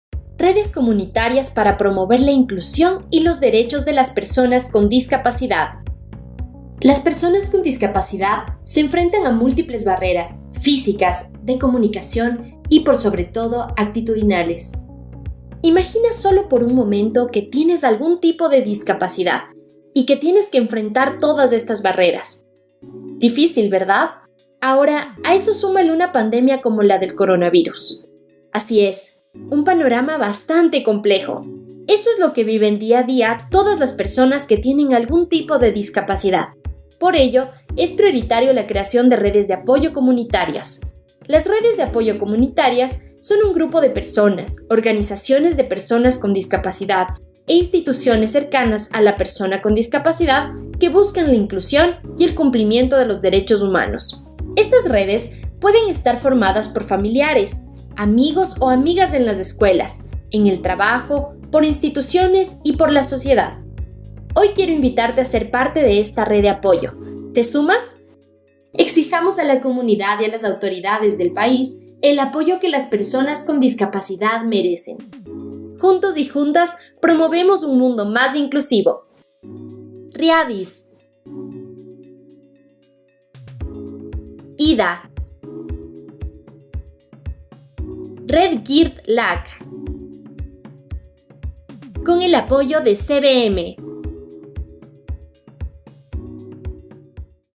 Redes comunitarias para promover la inclusión y los derechos de las personas con discapacidad
Source: Christian Blind Mission e.V. (CBM), RIADIS, International Disability Alliance (IDA), and Red GIRDD LAC